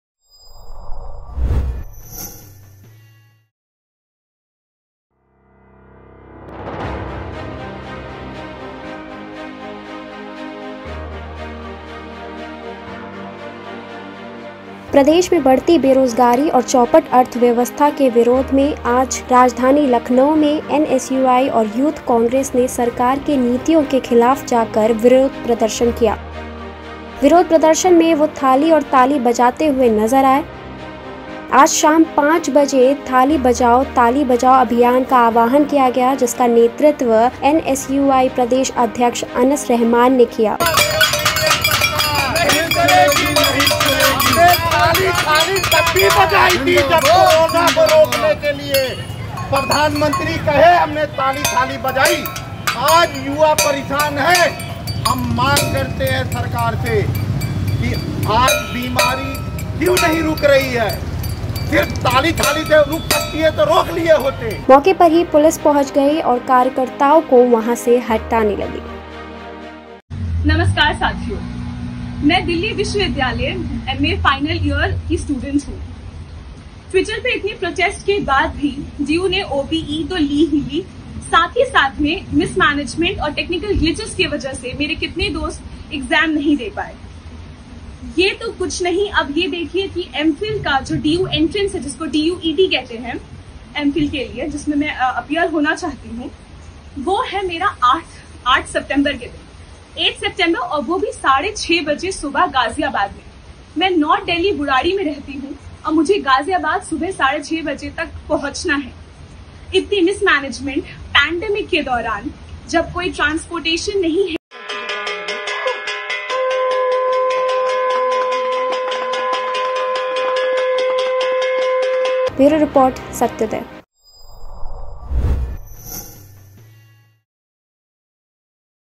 थाली बजाकर मांगी नौकरी
देश में बढ़ती बेरोजगारी, सरकारी नौकरियों की भर्ती प्रक्रिया में देरी और गिरती अर्थव्यवस्था को लेकर आज शाम 5 बजे कांग्रेस के छात्र संगठन एनएसयूआई और युवा कांग्रेस ने थाली बजाकर विरोध प्रदर्शन किया। कांग्रेस के इस थाली बजाओ अभियान को देश भर के युवाओं का समर्थन मिला। सोशल मीडिया पर 5Baje5Minute ट्रेंड करने लगा। एनएसयूआई के प्रदेश अध्यक्ष अनस रहमान के नेतृत्व में लखनऊ के तमाम क्षेत्रों में छात्रों ने शाम पांच बजे थाली बजाकर भाजपा सरकार की नीतियों के खिलाफ विरोध दर्ज कराया। लखनऊ विश्वविद्यालय में भी छात्रों ने थाली बजाई।
कांग्रेस प्रदेश अध्यक्ष ने थाली बजाते हुए कहा, देश और प्रदेश में भयंकर रूप से बढ़ती बेरोजगारी और लगातार गिरती अर्थव्यवस्था से देश के युवाओं का भविष्य खतरे में है। छात्र और युवा हताश व निराश हैं। जब युवा रोजी-रोटी की चिंता में नौकरी मांगता है, रूकी हुई परीक्षाएं कराने और अटकी भर्तियां घोषित करने की मांग करता है तो देश के प्रधानमंत्री कुत्ता पालने, खिलौना बनाने, पकौड़ा बनाने की सलाह देते हैं। साफ है कि प्रधानमंत्री का शिक्षा और युवाओं के भविष्य से कोई सरोकार नहीं रह गया है।
एनएसयूआई और यूथ कांग्रेस ने बेरोजगारी और चौपट अर्थव्यवस्था के विरोध में थाली ताली बजाकर सरकार की नीतियों का विरोध किया। राजधानी लखनऊ के निशातगंज में एनएसयूआई प्रदेश अध्यक्ष अनस रहमान के नेतृत्व में प्रदर्शन किया गया। इस मौके पर भारी संख्या में पहुंची पुलिस ने कार्यकर्ताओं को जबरन हटा दिया।